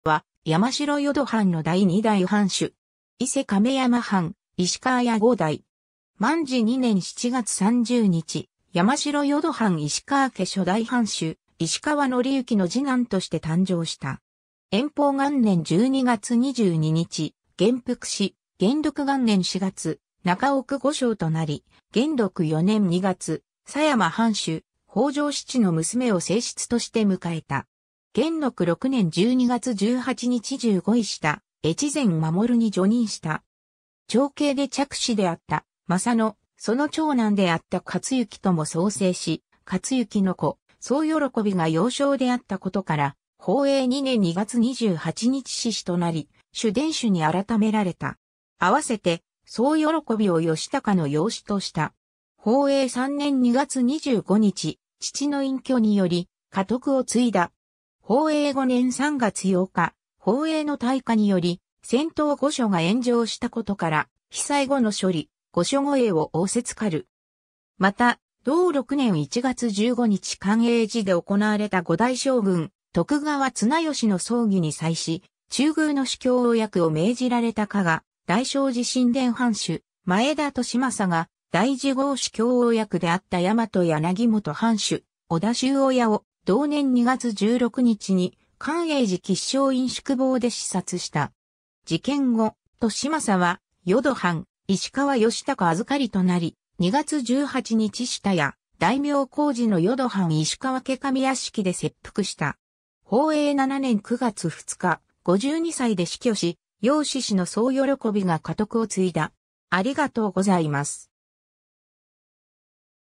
石川義孝
石川 義孝（いしかわ よしたか）は、山城淀藩の第2代藩主。
伊勢亀山藩石川家5代。
 万治2年7月30日（1659年9月16日）、山城淀藩石川家初代藩主・石川憲之の次男として誕生した。
延宝元年12月22日（1674年1月28日）、元服し、元禄元年4月（1688年5月）中奥小姓となり、元禄4年2月（1691年3月）、狭山藩主・北条氏治の娘を正室として迎えた。
元禄6年12月18日（1694年1月13日）従五位下・越前守に叙任した。
長兄で嫡子であった昌能、その長男であった勝之とも早世し、勝之の子・総慶が幼少であったことから、宝永2年2月28日（1705年3月23日）嗣子となり、主殿守に改められた。
併せて総慶を義孝の養子とした。
宝永3年2月25日（1706年4月8日）、父の隠居により家督を継いだ。
 宝永5年3月8日（1708年4月28日）、宝永の大火により仙洞御所が炎上したことから、被災後の処理・御所護衛を仰せつかる。
また、同6年1月15日（1709年2月24日）寛永寺で行われた5代将軍・徳川綱吉の葬儀に際し、中宮使饗応役を命じられた加賀大聖寺新田藩主・前田利昌が、大准后使饗応役であった大和柳本藩主・織田秀親を同年2月16日（3月26日）に寛永寺吉祥院宿坊で刺殺した。
事件後、利昌は淀藩石川義孝預かりとなり、2月18日（3月28日）下谷大名小路の淀藩石川家上屋敷（現東京都台東区上野1丁目）で切腹した。
 宝永7年9月2日（1710年10月23日）、52歳で死去し、養嗣子の総慶が家督を継いだ。